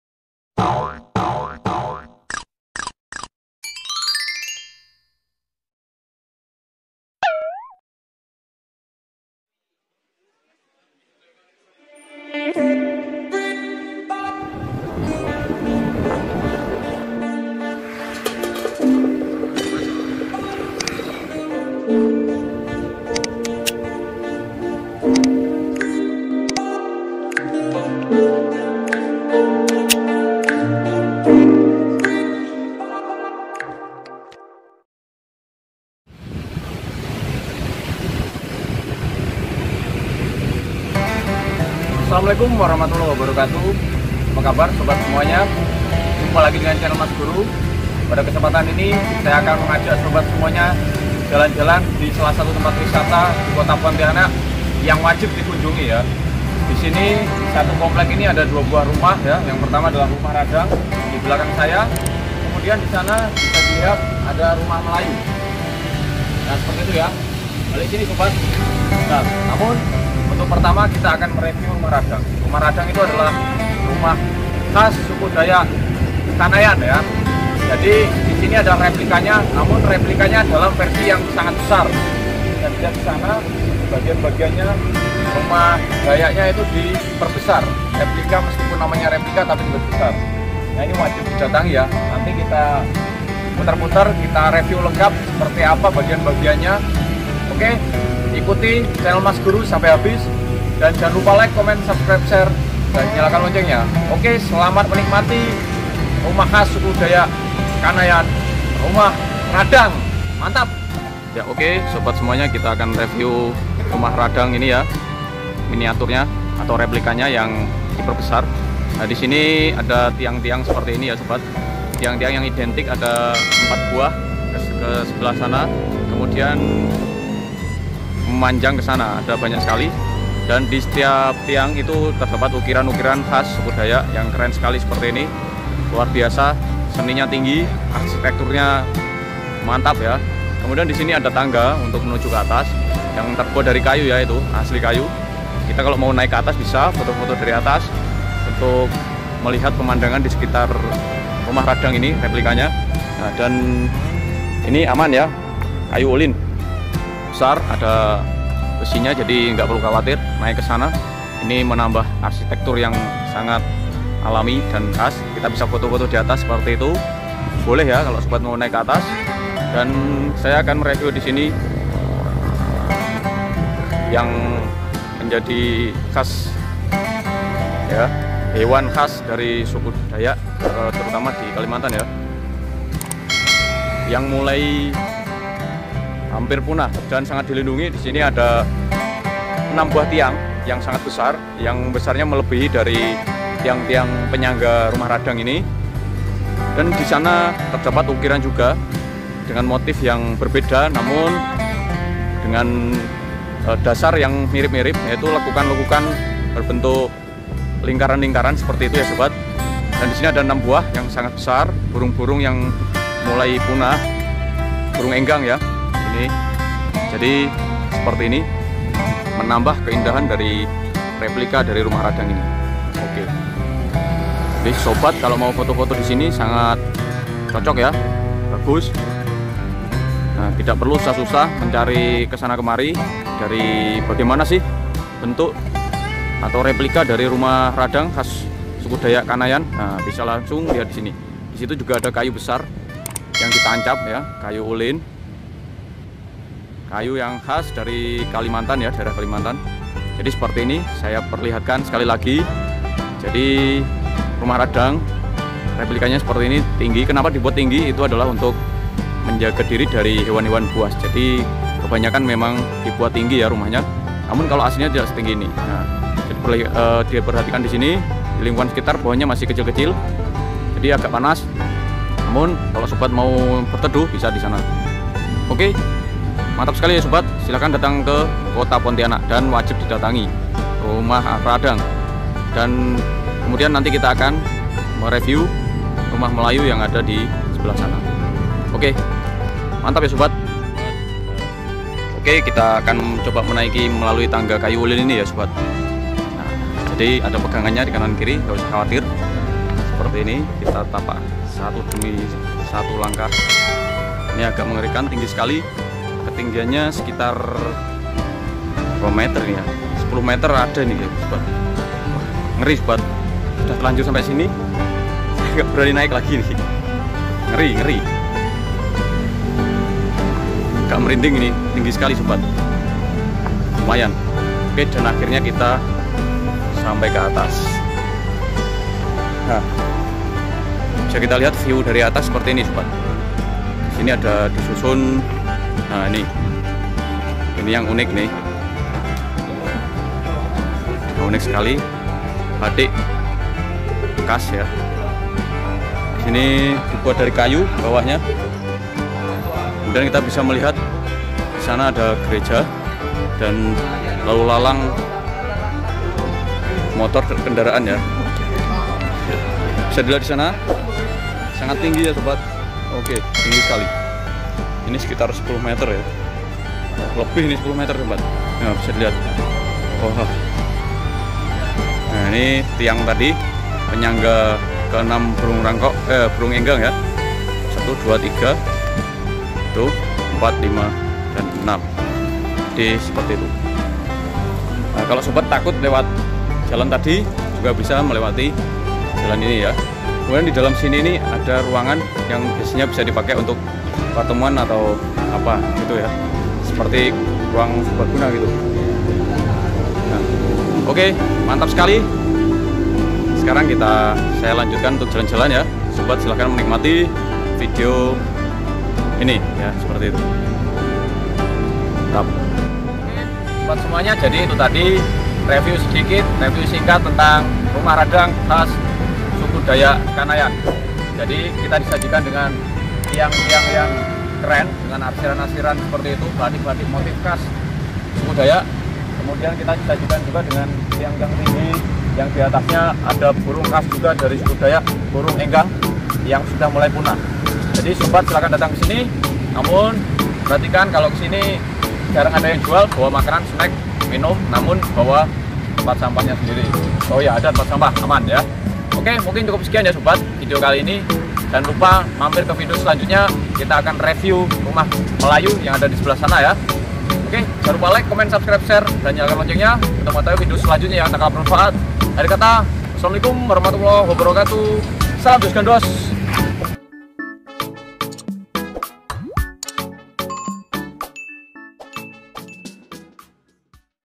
Rumah Radakng Khas Suku Dayak Kanayatn, Pontianak, Kalimantan Barat‼️Luar Biasa Rumah Khas Pontianak
Rumah Radakng merupakan rumah panjang yang menjadi rumah adat bagi Suku Dayak yang berada di Provinsi Kalimantan Barat. Ini merupakan rumah adat terbesar yang ada di Indonesia dan menjadi sebuah landmark bagi kota Pontianak setelah Tugu Khatulistiwa. Rumah Radakng memiliki ukuran panjang 138 meter dengan tinggi 7 meter. Lokasinya berada di Jalan Sutan Syahrir Kota Baru Pontianak.

Lokasi Rumah Radakng

Letaknya yang berada di Komplek Perkampungan Budaya di Jalan Sutan Syahrir Kota Baru Pontianak menjadikan rumah radakng sebagai ikon baru kota Pontianak setelah Tugu Khatulistiwa. Rumah adat raksasa tersebut telah diresmikan oleh Gubernur Kalimantan Barat, pada tahun 2013 silam. Dalam peresmiannya tersebut disampaikan bahwa rumah adat yang menjadi kebanggaan Provinsi Kalimantan Barat bisa digunakan untuk pengembangan dan pelestarian adat istiadat serta sebagai ikon pariwisata di Kalimantan Barat. Dengan demikian, kehadiran rumah radakng ini memberikan arti positif bagi pengembangan pariwisata di Pontianak dan juga Kalimantan Barat secara umum. Pemanfaatannya bisa untuk kegiatan seni dan budaya yang tentunya akan menarik wisatawan untuk terus berkunjung. Para wisatawan yang berkunjung, nantinya akan disiapkan berbagai macam suvenir yang berupa miniatur dari rumah adat tersebut. Rumah adat ini mampu menampung 600 orang di ruang utamanya. Sementara untuk halamannya juga didesain cukup luas untuk digunakan berbagai aktivitas budaya lainnya. Rumah adat raksasa kebanggaan masyarakat Dayak ini terbuat dari kayu yang ukurannya sangat besar dan panjang. Sangat megah dan indah dipandang.